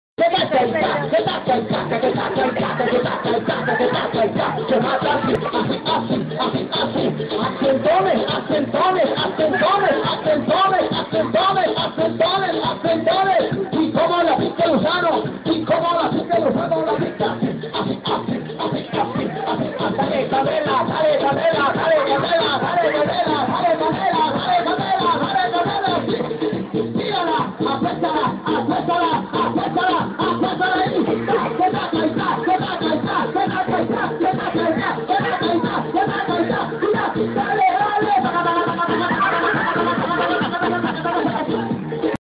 doctor simi matando el gusano en culiacan
esa pick esta chida y el doctor simi baila asi y como se maya el gusano hehehehe jejejejejeje